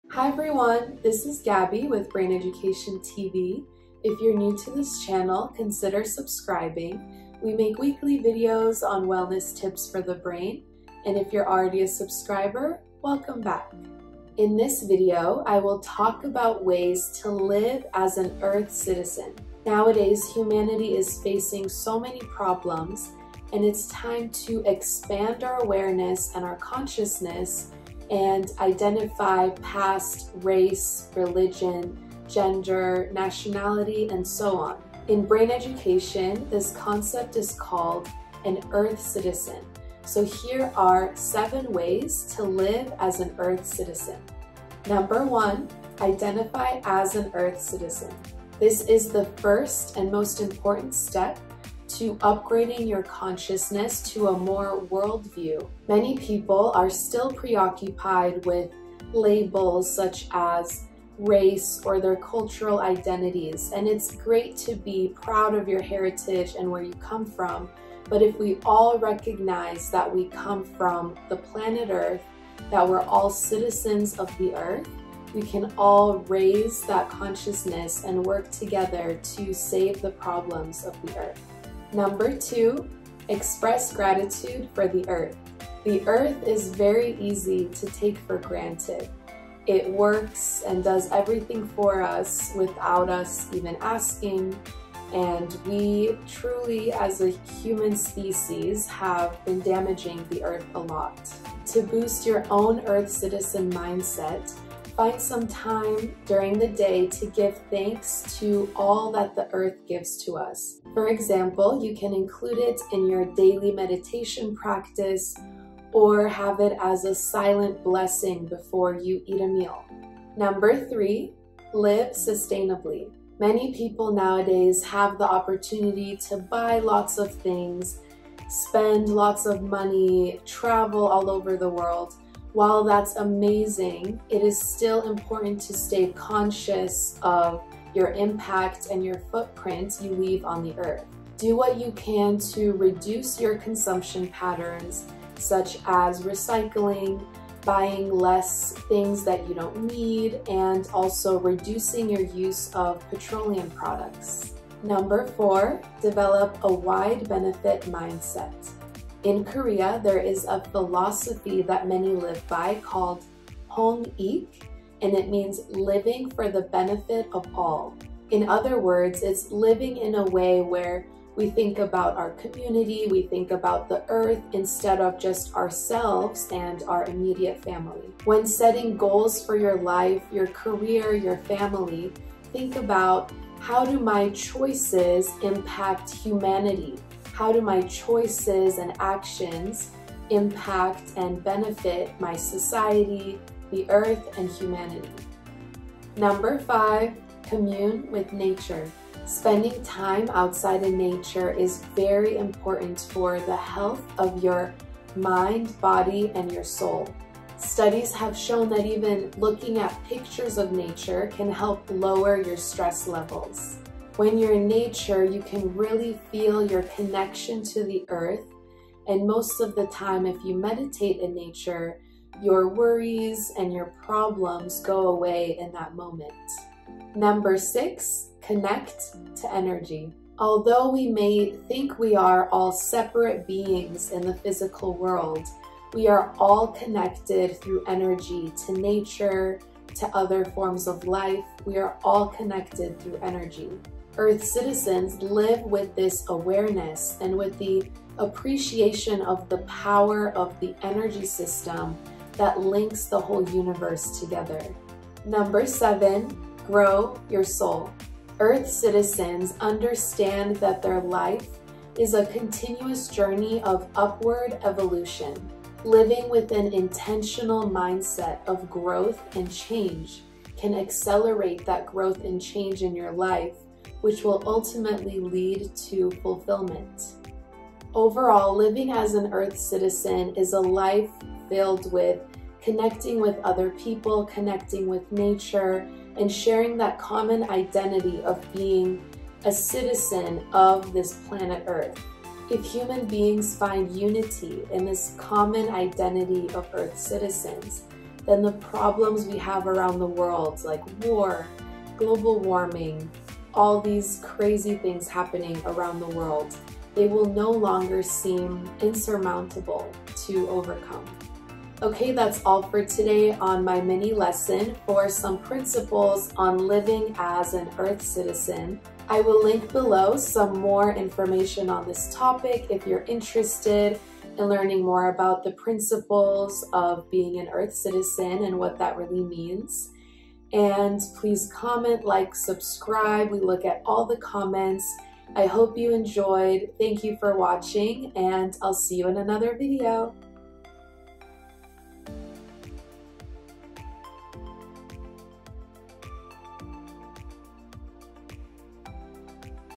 7 Ways to Live an Earth Citizen Lifestyle | Live to Save the Earth and Humanity
When you take away labels of race, class, gender, nationality, and so on, we are all citizens of the Earth. If we come together and live as Earth Citizens, we can start to solve some issues that the Earth and humanity is facing. In this video, learn 7 ways how you can live as an Earth Citizen.